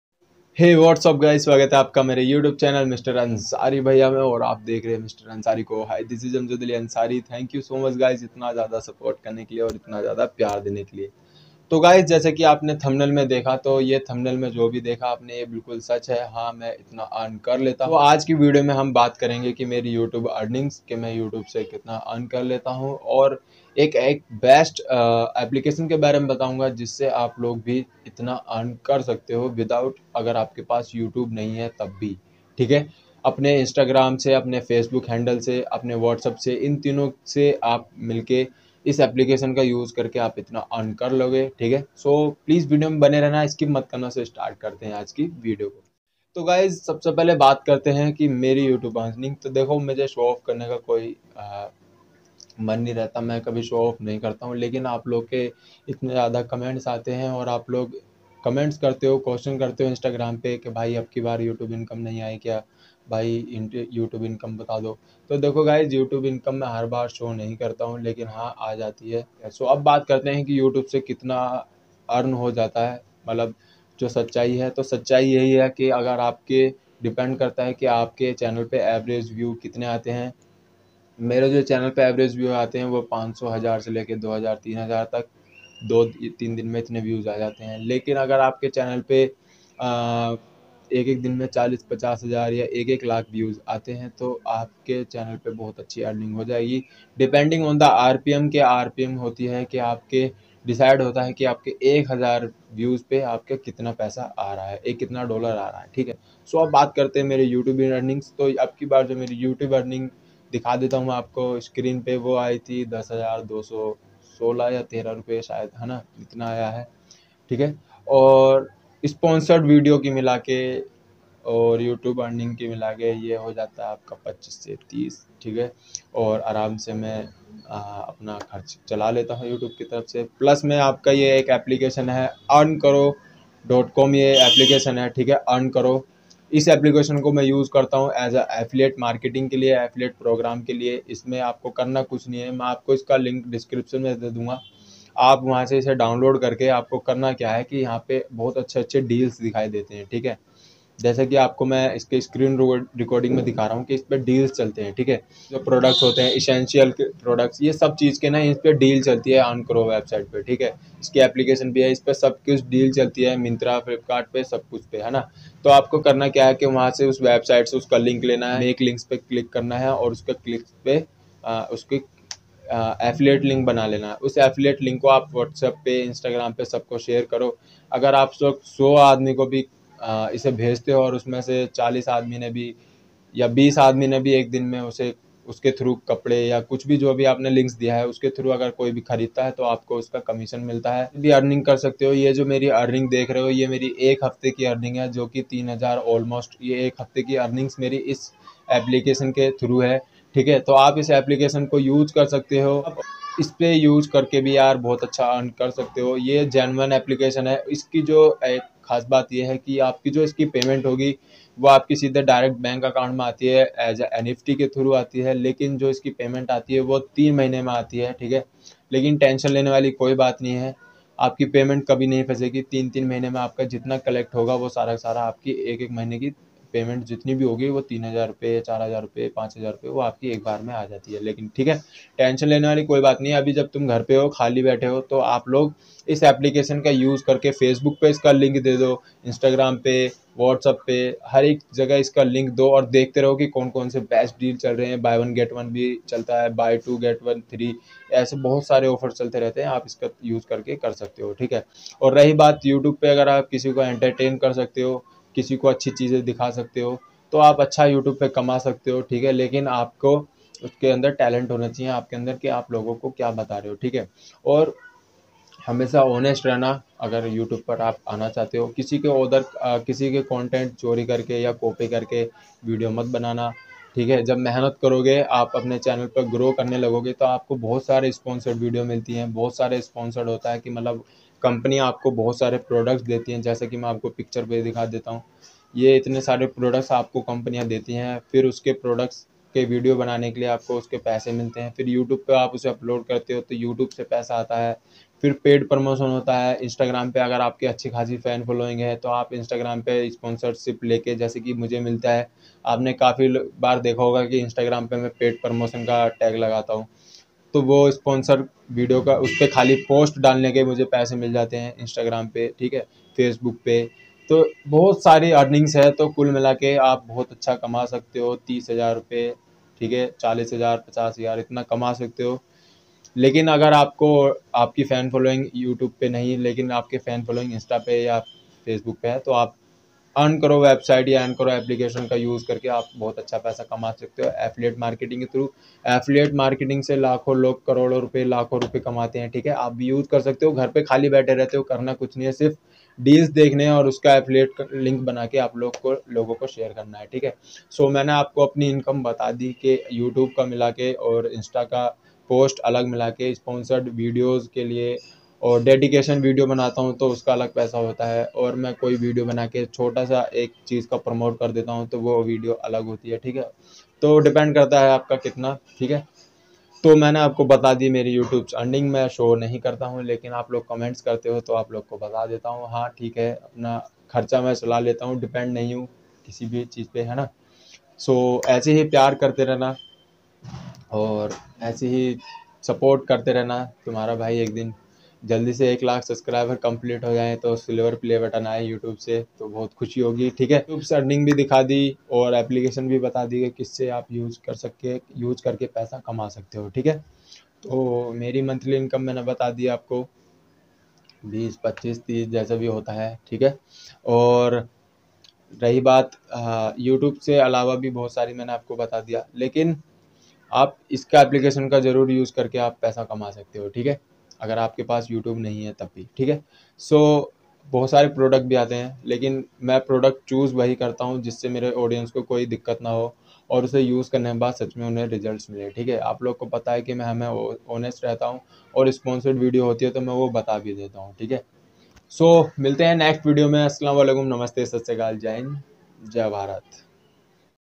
गरीब परिवार से हूं तो क्या हुआ सपने तो पूरे कर रहा हूं।my YouTube earnings 2022 । mr.ansari bhaiya
My first payment from youtube 2022 |my success story| youtube से कितना कमाता हूं |mr.ansari bhaiya
hii this is Amjad Ali Ansari.

Friends, I always try that my videos are different from others' videos, so I always do research and make videos